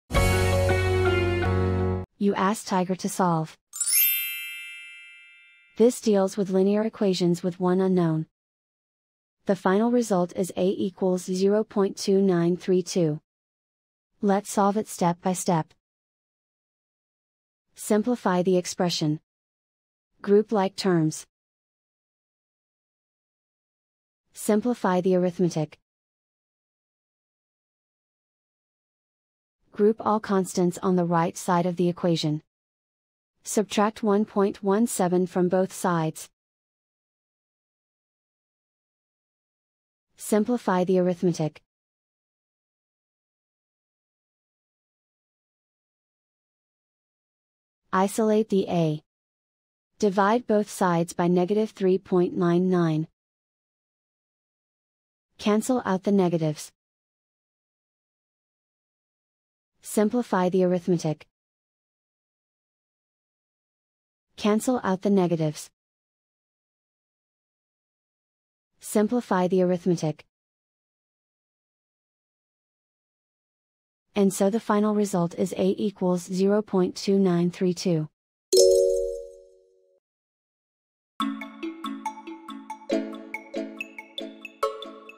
Linear equation with one unknown: Solve 1.17-0.07a+(-3.92a)=0 step-by-step solution
Solving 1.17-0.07a+(-3.92a)=0 using the linear equation with one unknown solver

for a step-by-step and more tips and tricks for dealing with linear equations with one unknown.